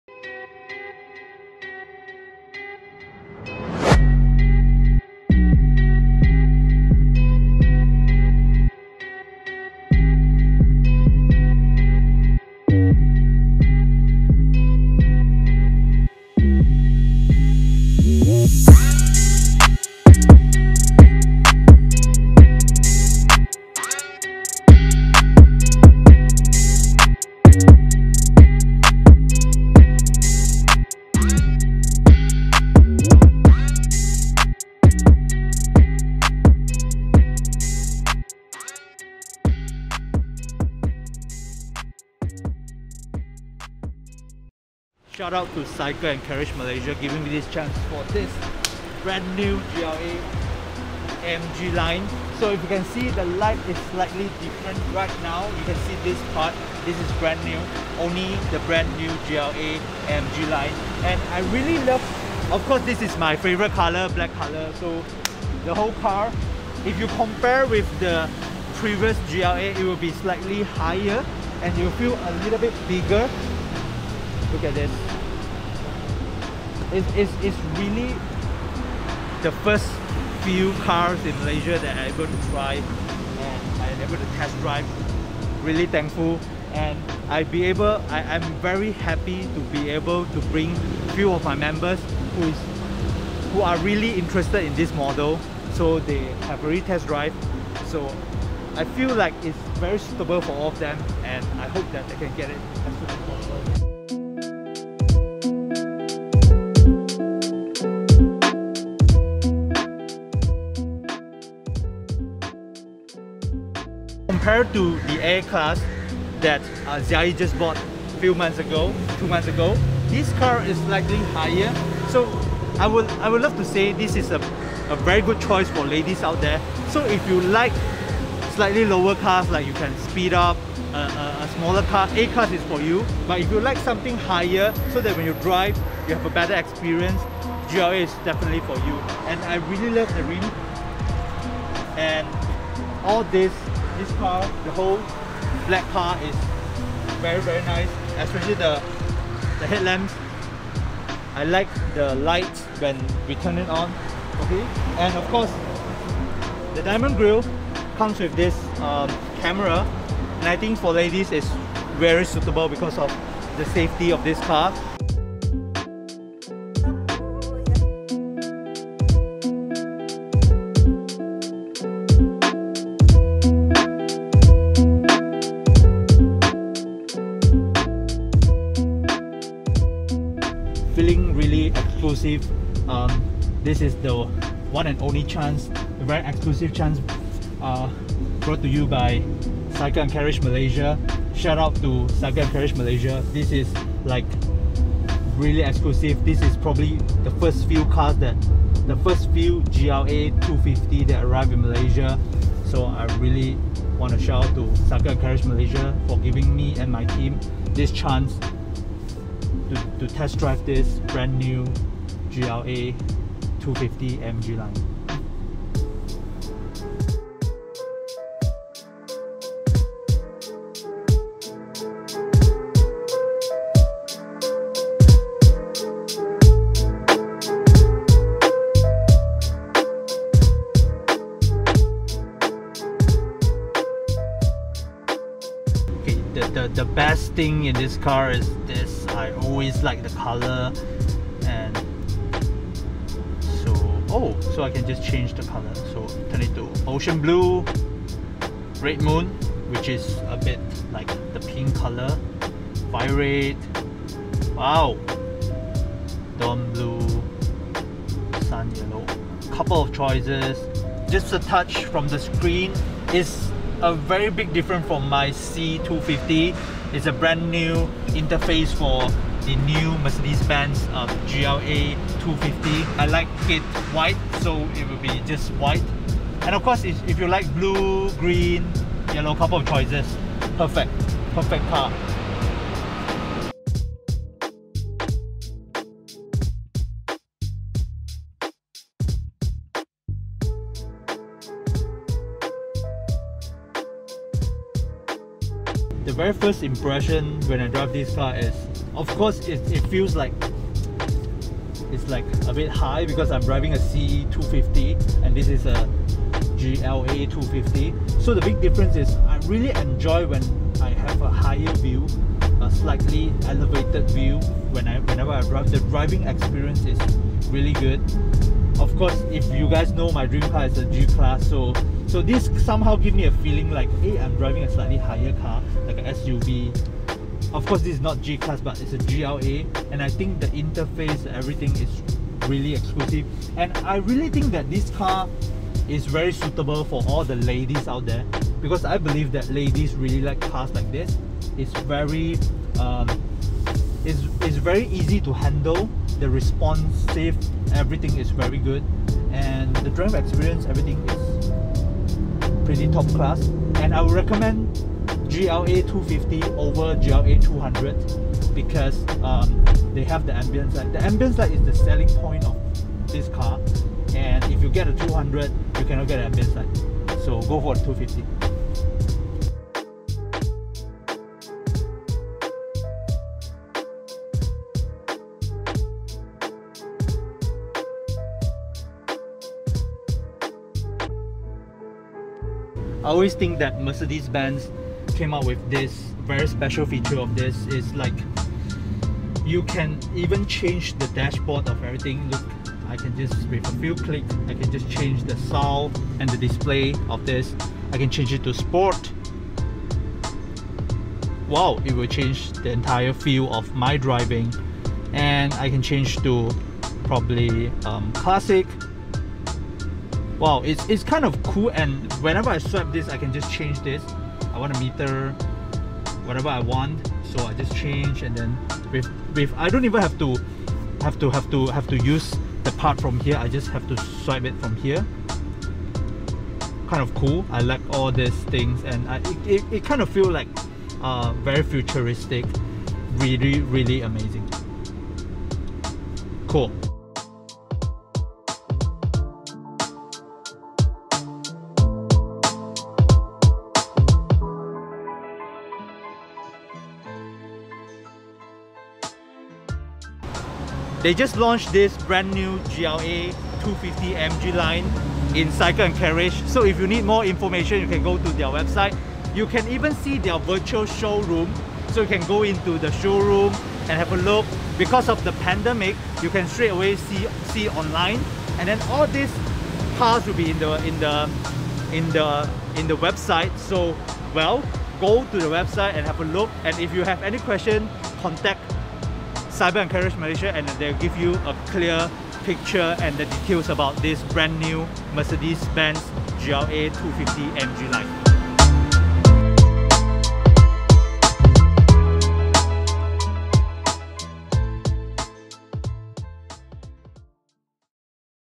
MERCEDES BENZ GLA 250 MALAYSIA AMG LINE | CYCLE & CARRIAGE EXCLUSIVE PREVIEW | SHENGYI KOH EP24
CYCLE & CARRIAGE MALAYSIA EXCLUSIVE PREVIEW FOR SHENGYI AND BELIEVE TEAM TOP PRODUCERS WHO ARE INTERESTED TO THE BRAND NEW MERCEDES BENZ GLA 250 AMG LINE.

THANK YOU CYCLE & CARRIAGE FOR THE EXCLUSIVE REVIEW!